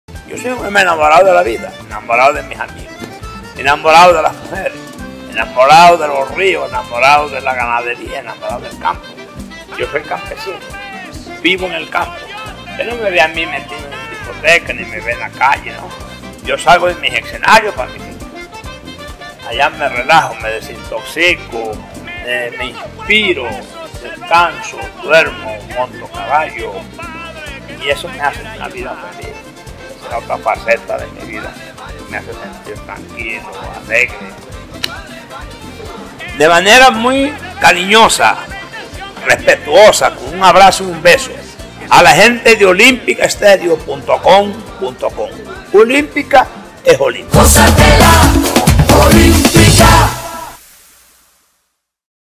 Así se define Poncho Zuleta
Tomás Alfonso ‘Poncho’ Zuleta Díaz se define tal cual y como es para los visitantes de